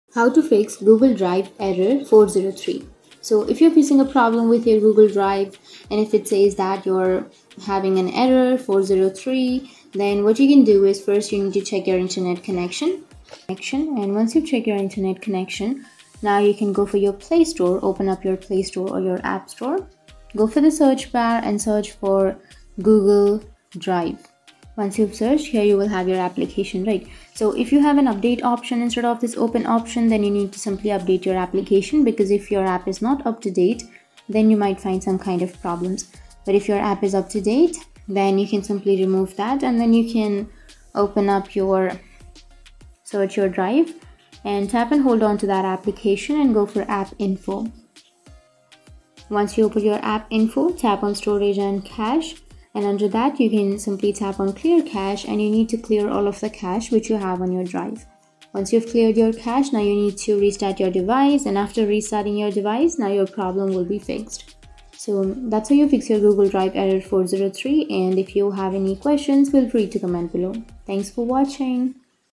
How To Fix Google Drive Error 403 !! Easy Guide
How To Fix Google Drive Error 403

Experiencing Google Drive Error 403? Don't worry, MiniProTips has got you covered! In this quick tutorial, learn effective methods to troubleshoot and resolve the Google Drive Error 403.

Discover step-by-step instructions to address common causes of the error, including permission issues, server problems, or incorrect configurations. With MiniProTips, fix Google Drive Error 403 swiftly and get back to accessing your files seamlessly on Google Drive.